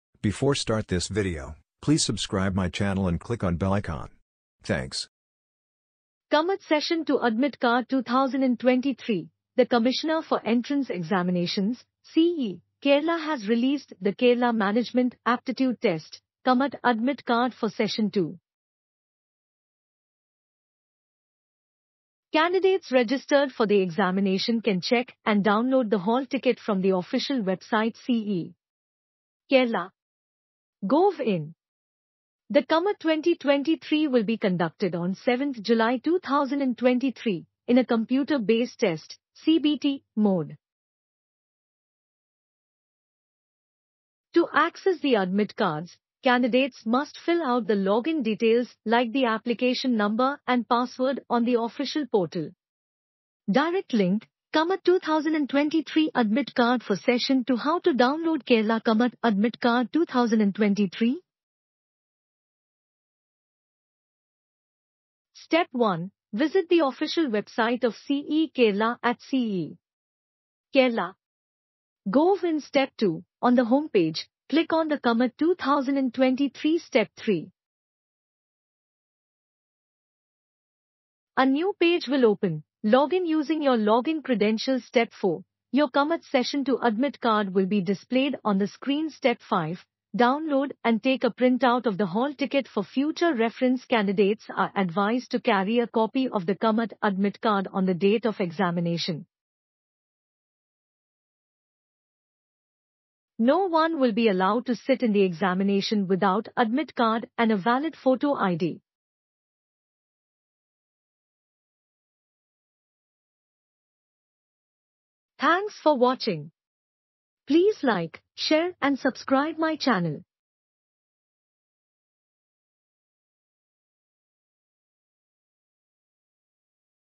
Kerala KMAT Admit Card 2023 for Session 2 released on cee.kerala.gov.in, direct link here
KMAT Session 2 Admit Card 2023: The Commissioner for Entrance Examinations (CEE) Kerala has released the Kerala Management Aptitude Test, KMAT Admit Card for Session 2. Candidates registered for the examination can check and download the hall ticket from the official website cee.kerala.gov.in. The KMAT 2023 will be conducted on July 7, 2023, in a Computer Based Test (CBT) mode. To access the admit cards, candidates must fill out the login details like the application number and password on the official portal. Direct Link: KMAT 2023 Admit Card for Session 2 How to download Kerala KMAT Admit Card 2023 ?...